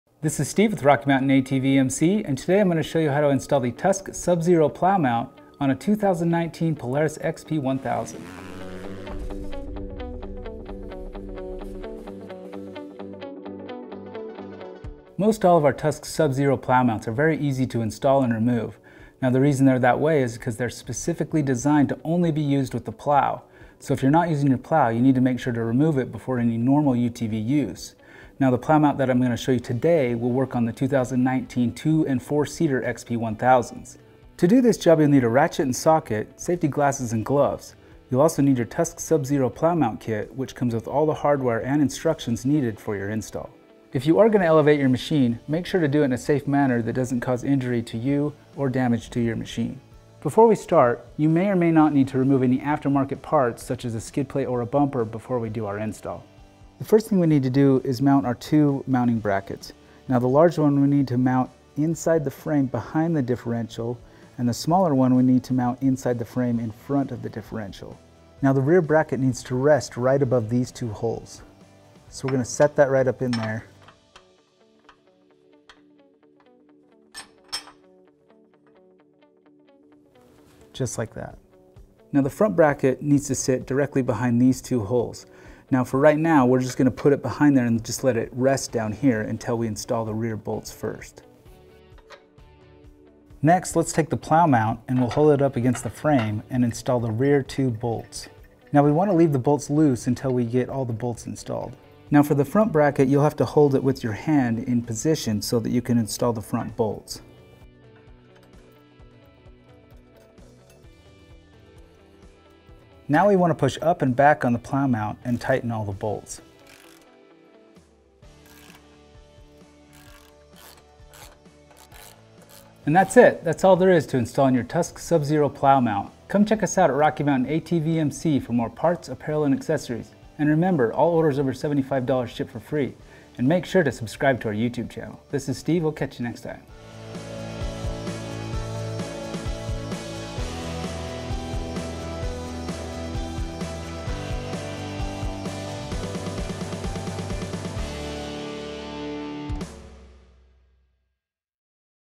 How To Install the Tusk SubZero Snow Plow Mount | Polaris RZR XP 1000
In this video we show you the proper steps to install the Tusk  SubZero Plow mount on a 2019 Polaris RZR XP 1000. The specific mount for your machine will be included with any Tusk SubZero Snow Plow Kit and is only available as a replacement.